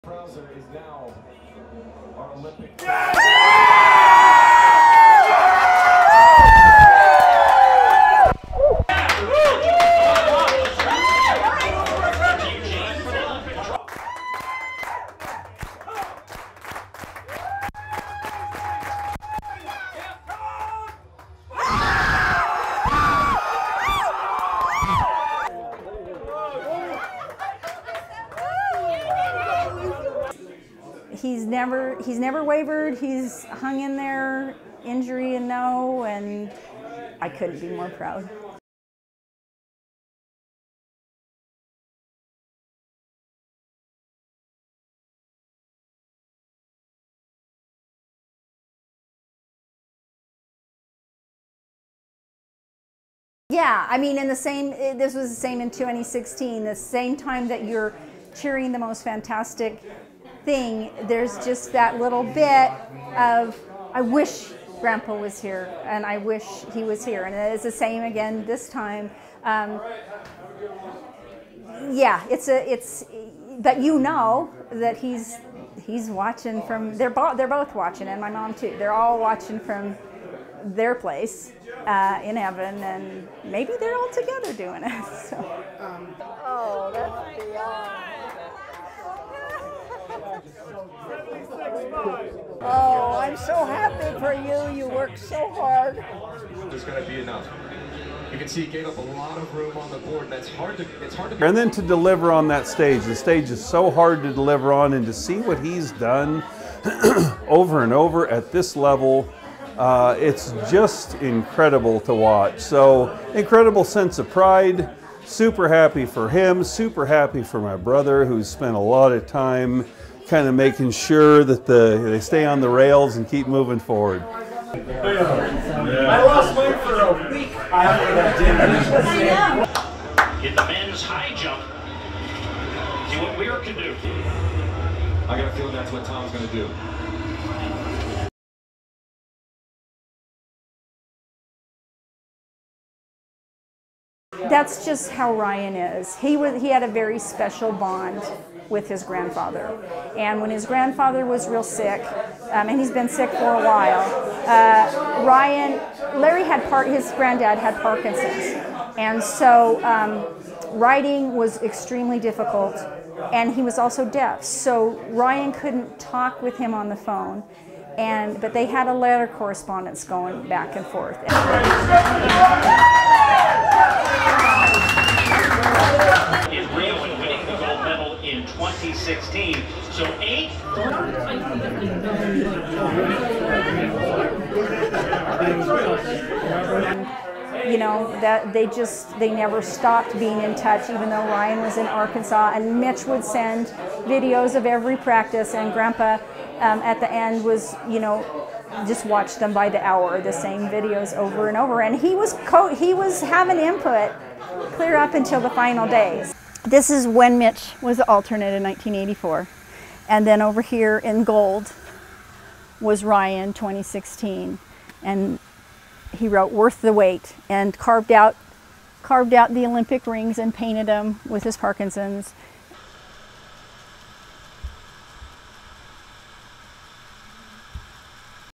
Elation as mom watches son clinch gold medal
(5 Aug 2021) The family of shot putter Ryan Crouser erupted into cheers at their house when he clinched the gold medal in Tokyo.
Crouser broke his own Olympic record on his way to defending his shot put title Thursday on a hot day in Tokyo.
"I couldn't be more proud," said his mother Lisa Crouser after watching with family and friends in her home in Redmond, Oregon.
On his last attempt, Crouser went 23.30 meters (76 feet, 5½ inches) to earn the first track and field gold medal for the American men at the Tokyo Games. U.S. teammate Joe Kovacs finished second and Tomas Walsh of New Zealand was third.
The 28-year-old Crouser went 22.52 meters when he won at the 2016 Rio de Janeiro Games.
Crouser is already the world-record holder after breaking a 31-year-old mark on June 18 at the U.S. Olympic trials. His attempt that evening went 23.37 meters.
In the stifling heat at the Olympic Stadium, Crouser took the lead on his first attempt and saved his best for his final one. He clapped his hands — sending chalk dust into the air — and celebrated. He even donned his cowboy hat for the occasion.
His family and friends gathered for a watch party. Wearing "Crouser" shirts, they let out the largest cheer of all when the commentators announced those magical words: "Gold Medalist."
He was competing with a heavy heart after his grandfather recently died. On a piece of paper, Crouser had a heartfelt message: "Grandpa. We did it. 2020 Olympic champion!"
"He was he had a very special bond with his grandfather," Lisa Crouser said, adding that the gold medalist would write letters to his grandfather suffering from Parkinson's disease and deafness.
Ryan Crousers' uncle, Brian, who was also an Olympian in Javelin, said his father, Larry, was the source of the family's love of sports.
"The stage is so hard to deliver on and just see what he's done over and over at this level. It's just incredible to watch. So incredible sense of pride, super happy for him, super happy for my brother, who spent a lot of time kind of making sure that they stay on the rails and keep moving forward," Brian Crouser said.